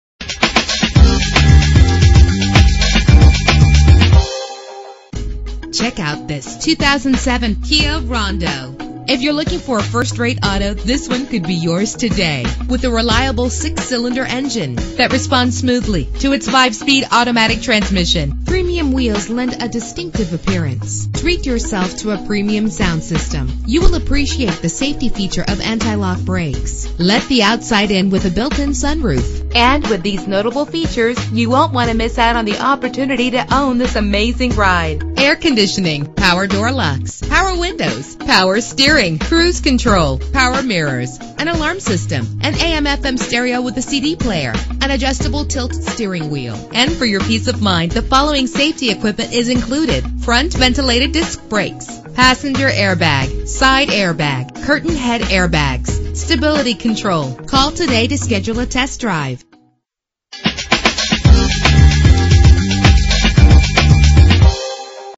2007 Kia Rondo - Ewing NJ
Year: 2007
Make: Kia
Model: Rondo
Trim: EX
Engine: 2.7 liter 6 cylinder 24 valve
Transmission: 5-Speed Automatic
Color: Blue
Mileage: 77765
Address: